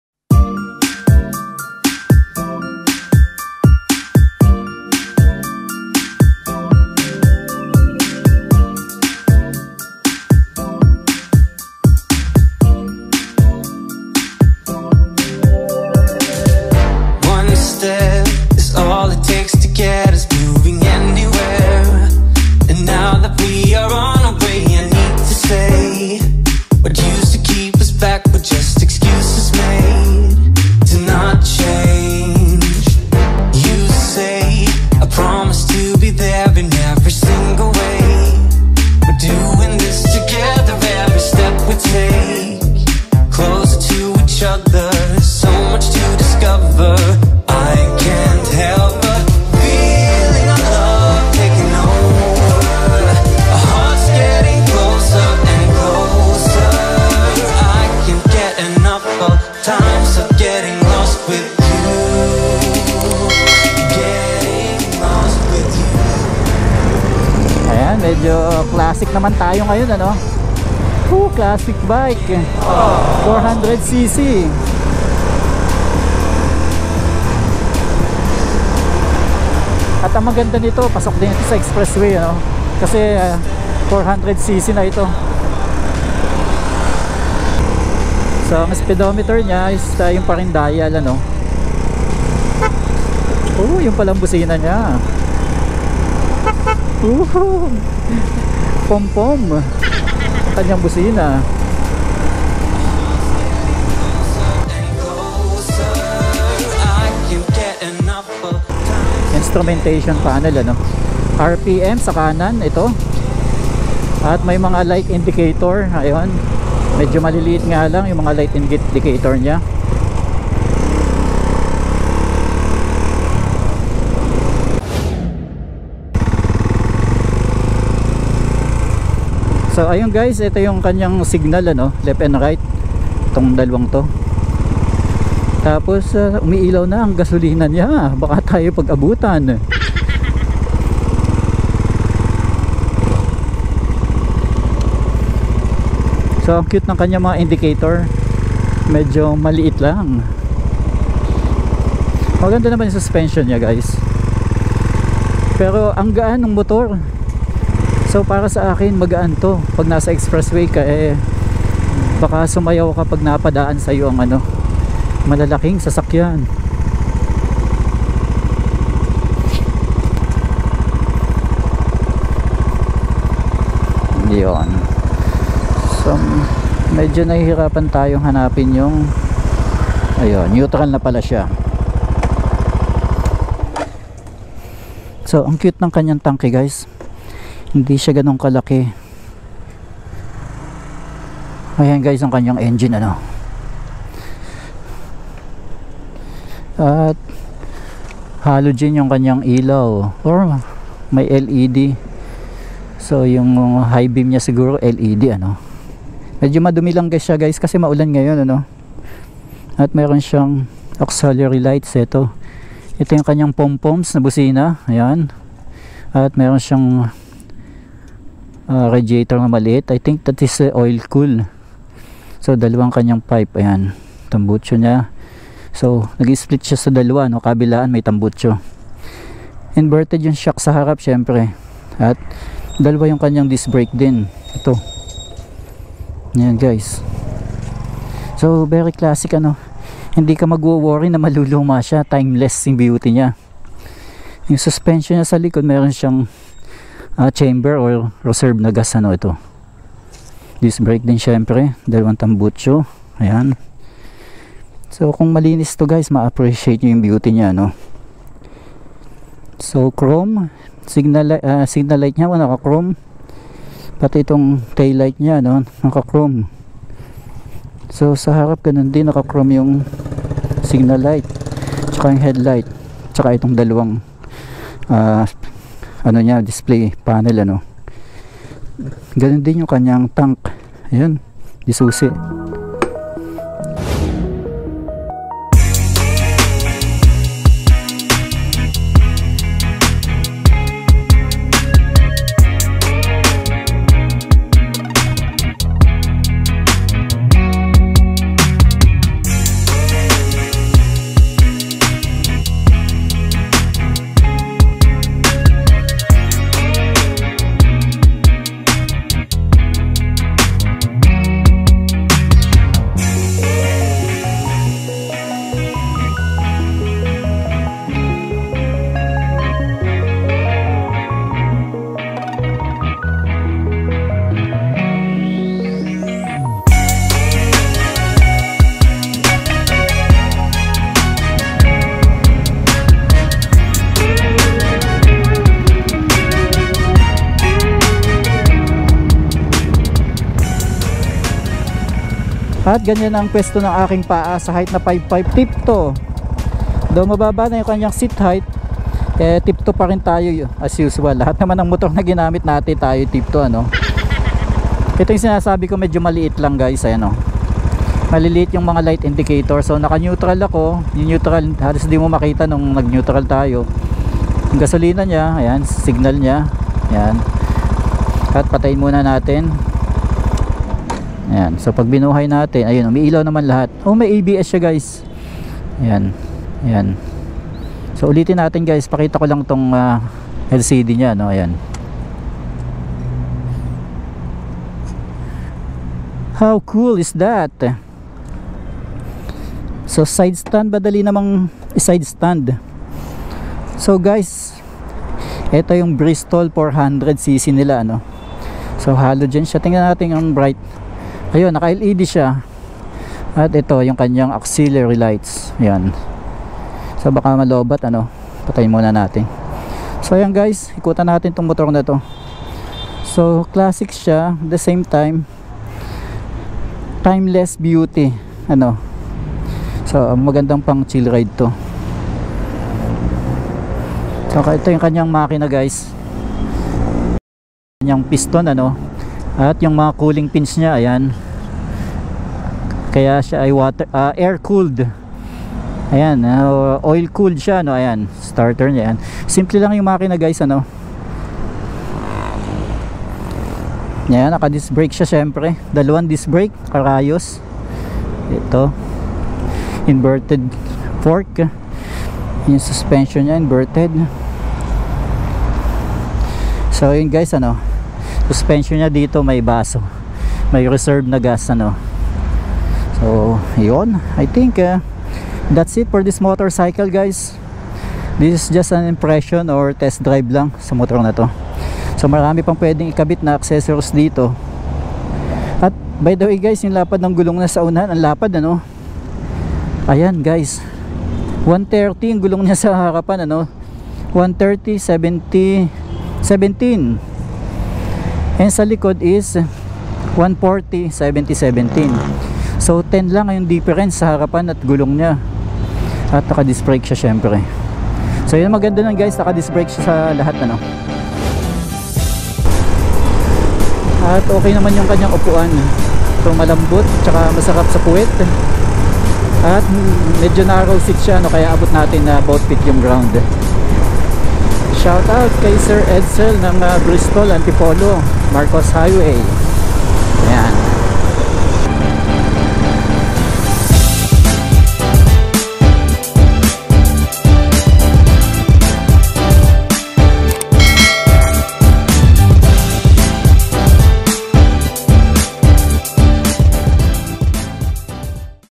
BRISTOL OMEGA 400i | Review | First Impressions | Test ride | Classic Bike
Bristol Omega 400i Full Review, Classic looking 400cc from Bristol Motorcycles Marcos Antipolo.

If you want to stay in touch on a personal level, get updates on my channel, and just want to be friends.

GoPro Hero8 2x (Dual Cam Setup)
GoPro Max 360
DJI Mini2
Sony Alpha 6000
Apple iPhone 11 Promax 256gb

Timecodes
00:00 -teaser
00:49 -iamjamich intro
01:05 -first impressions
02:59 -overview
04:44 -test ride teaser
05:41-seat height
05:57-quick review
08:43 -iamjamich Outro